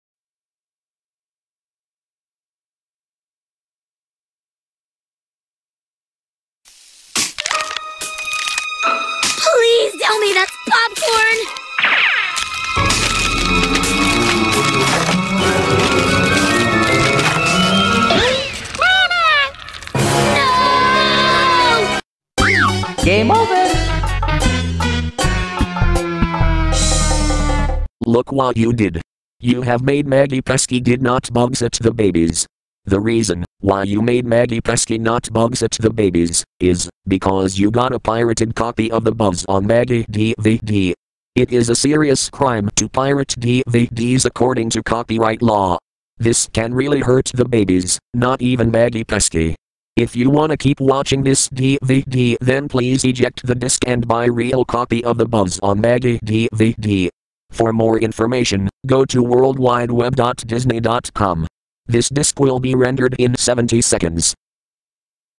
The Buzz on Maggie DVD Anti-Piracy Screen (FAKE)
Hey everyone, this is an Anti-Piracy Screen that I decided to do. And also, do not take down my videos! Cause if you do, then I will be mad!